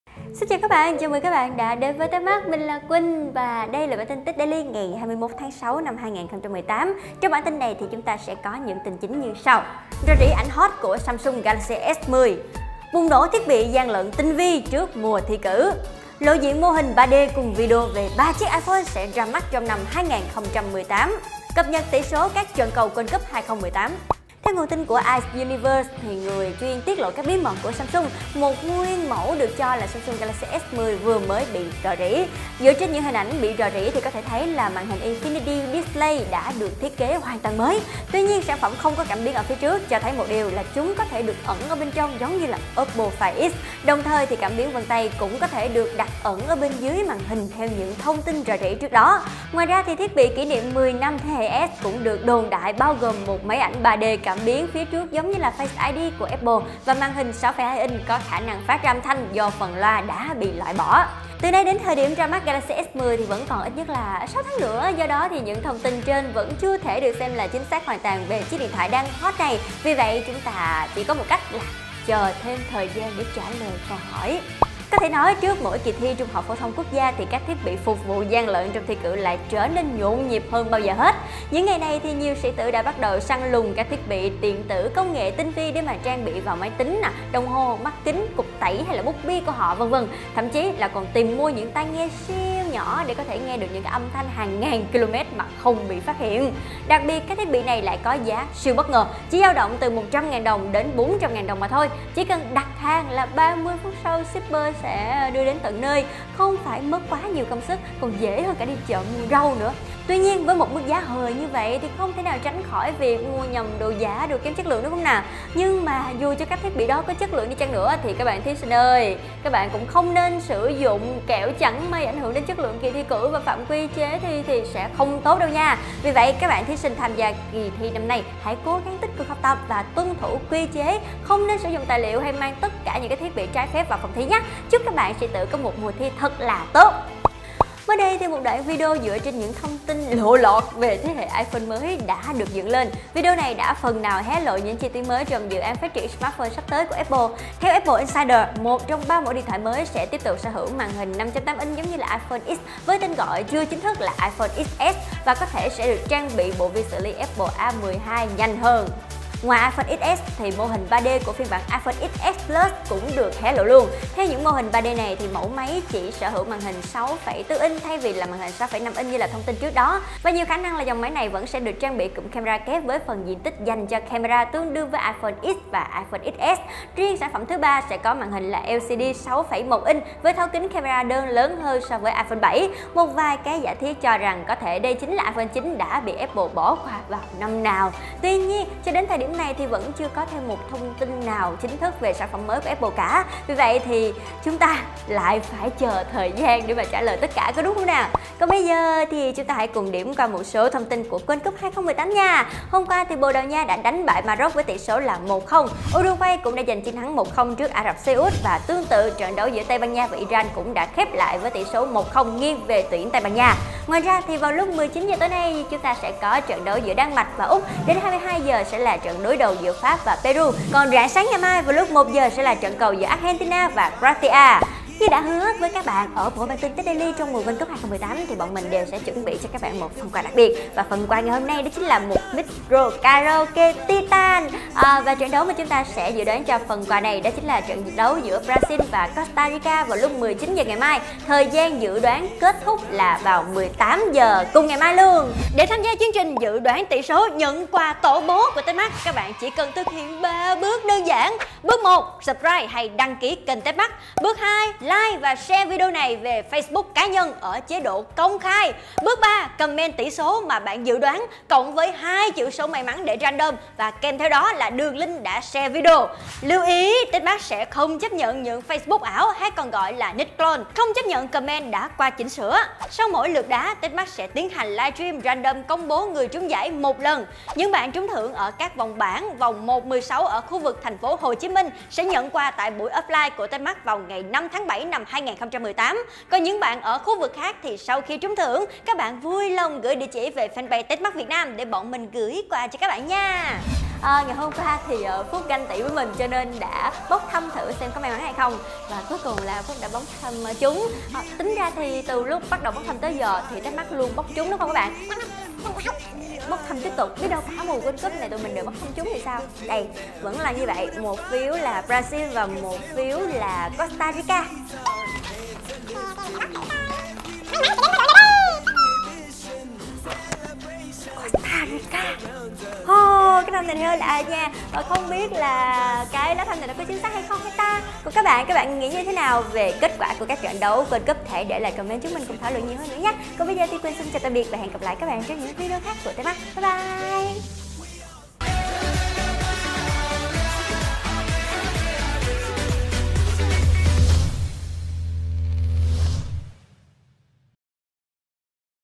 [Tặng quà] Galaxy S10 lộ thiết kế
Trong bản tin Daily ngày 21/6 có những tin sau:
Rò rỉ hình ảnh “hot” của Samsung Galaxy S10
Bùng nổ thiết bị gian lận tinh vi trước mùa thi cử
Lộ diện mô hình 3D cùng video về 3 chiếc iPhone sẽ ra mắt trong năm 2018
Cập nhật tỉ số các trận cầu World Cup 2018